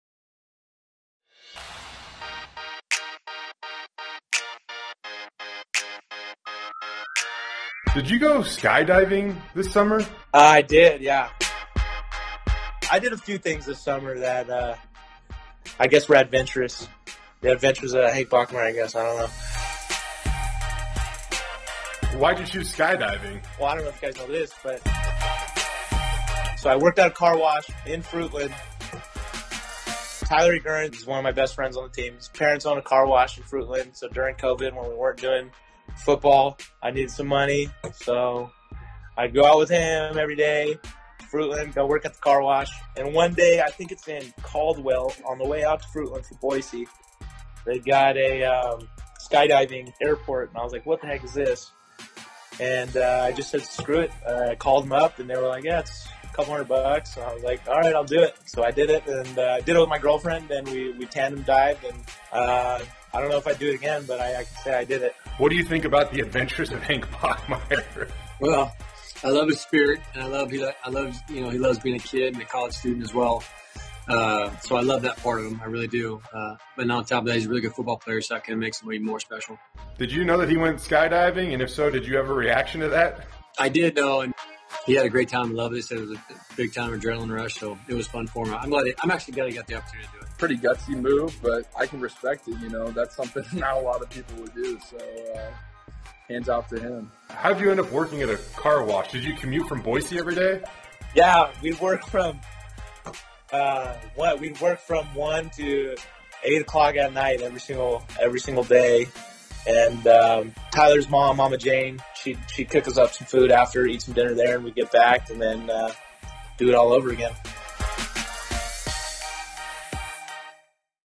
The adventures of Boise State quarterback Hank Bachmeier
Boise State quarterback Hank Bachmeier is fearless on and off the football field.
Hank and his girlfriend went skydiving in Caldwell last summer. It was big adrenaline rush.